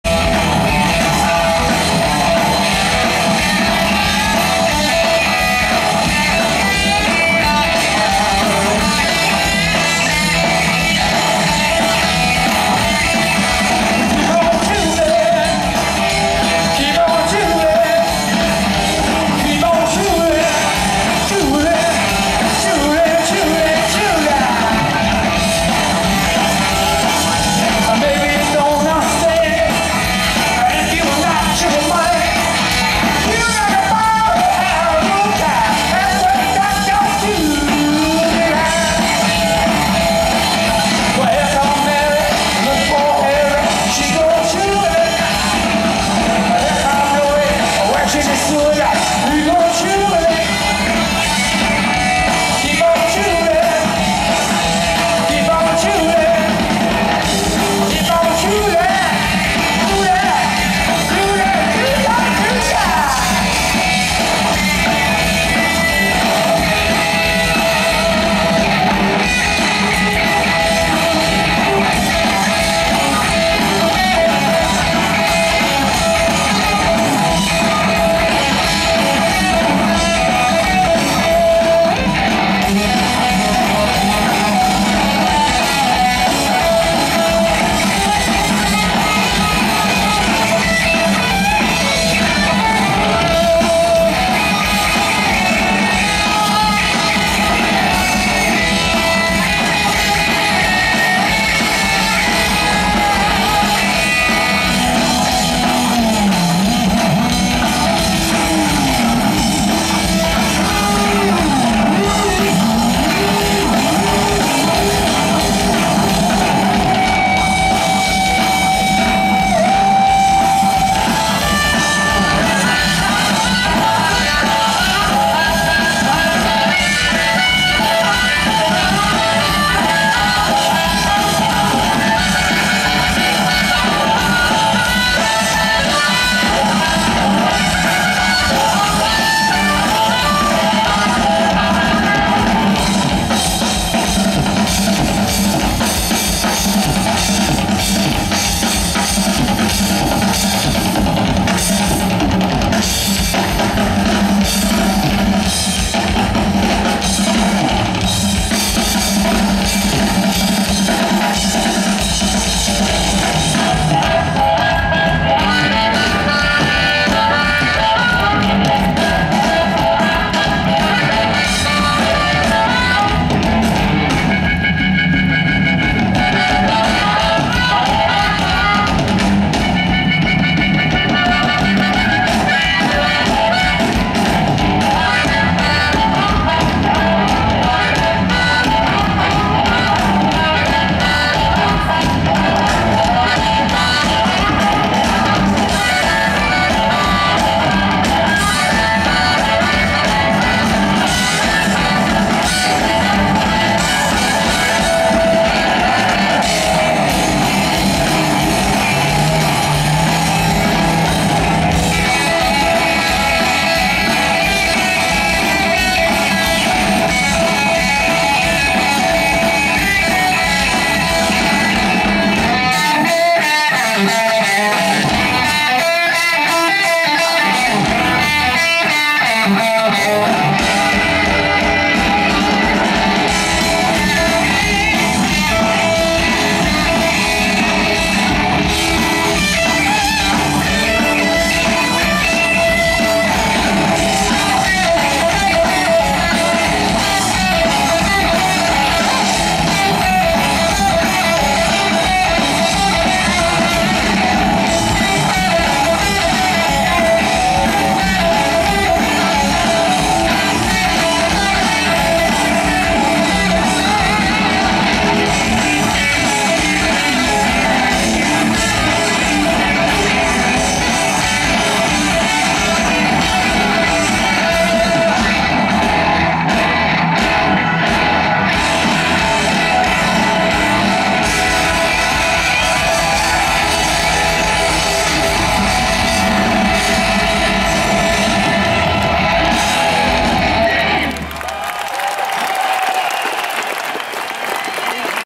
John Fogerty - @ Dalhalla, Sweden
6th July 2012, Dalhalla, Rättvik, Sweden
HD-QUALITY!